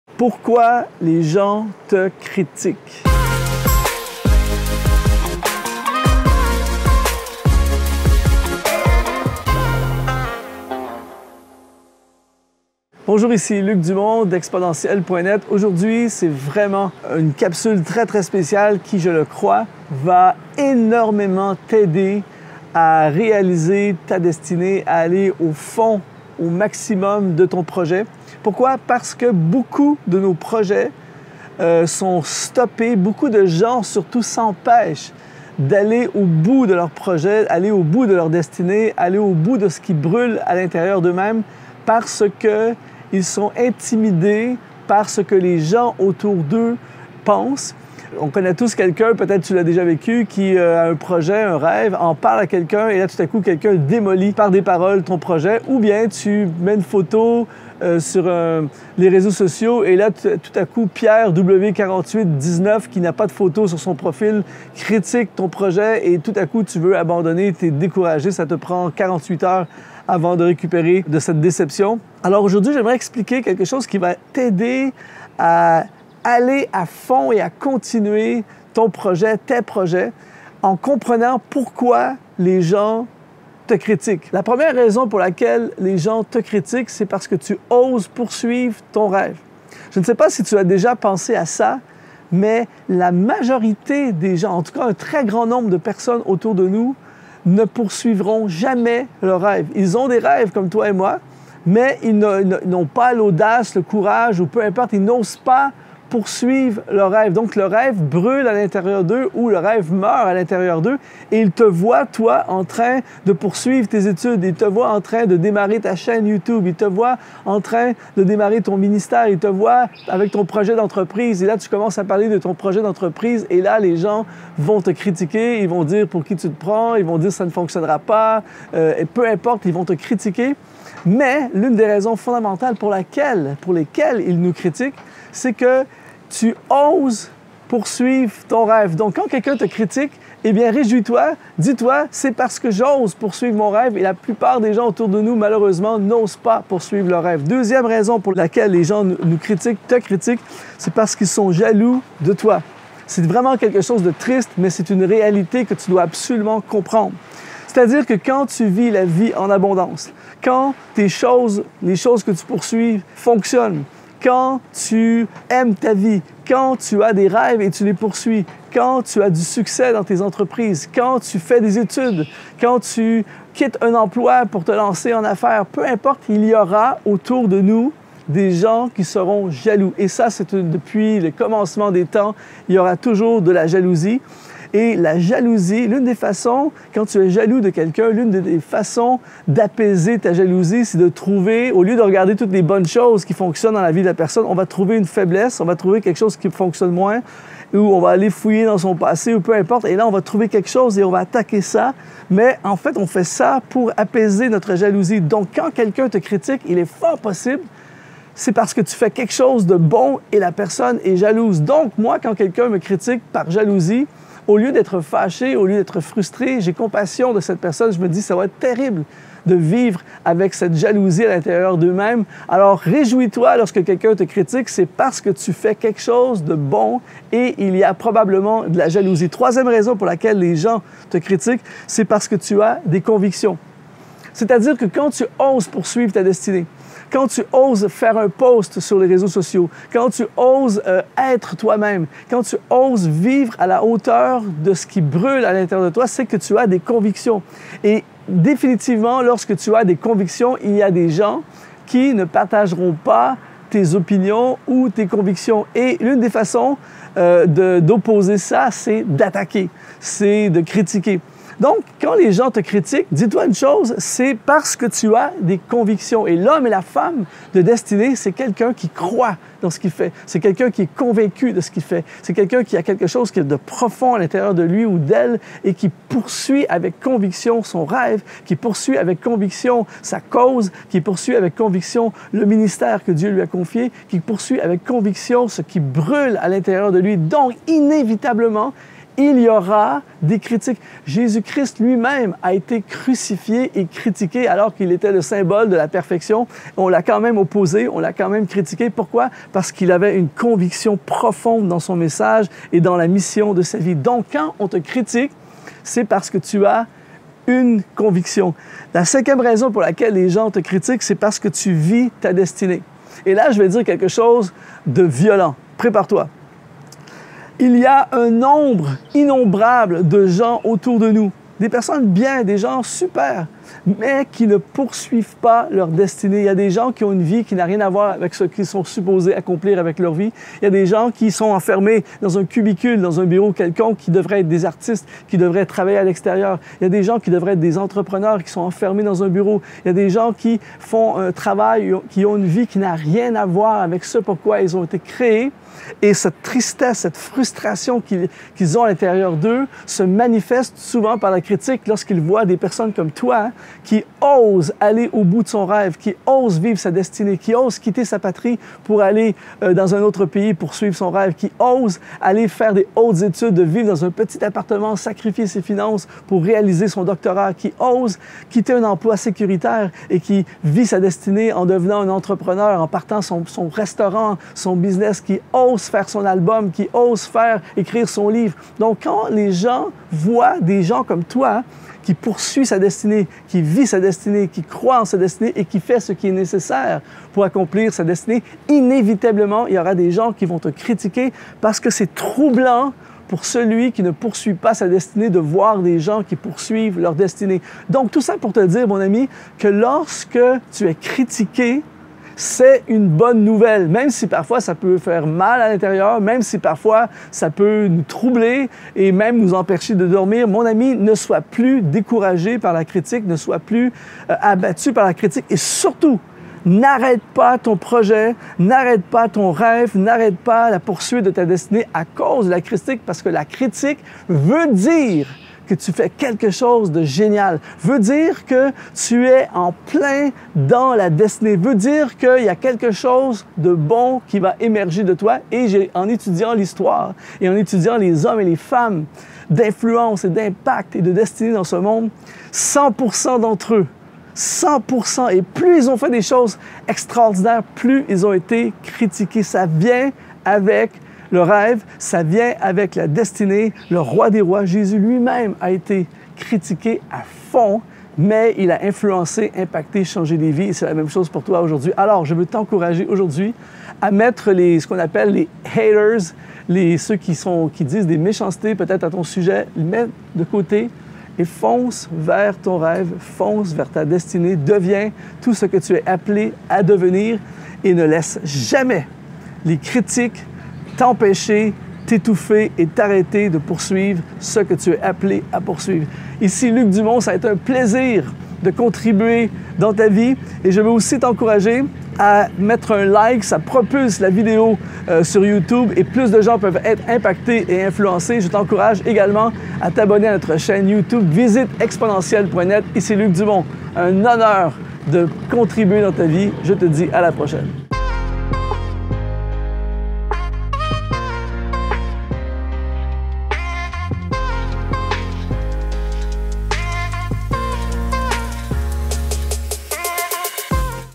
POURQUOI LES GENS TE CRITIQUENT - Luc Dumont
Plusieurs personnes se découragent et abandonnent leur projet à cause des critiques.
Dans cette capsule, j’explique pourquoi certains te critiquent. Après avoir vu cette vidéo, tu ne verras plus la critique de la même façon et tu seras encouragé.

➡️➡️ FORMATION GRATUITE avec Luc Dumont
         4 RÉPONSES POUR LE SUCCÈS DE TA DESTINÉE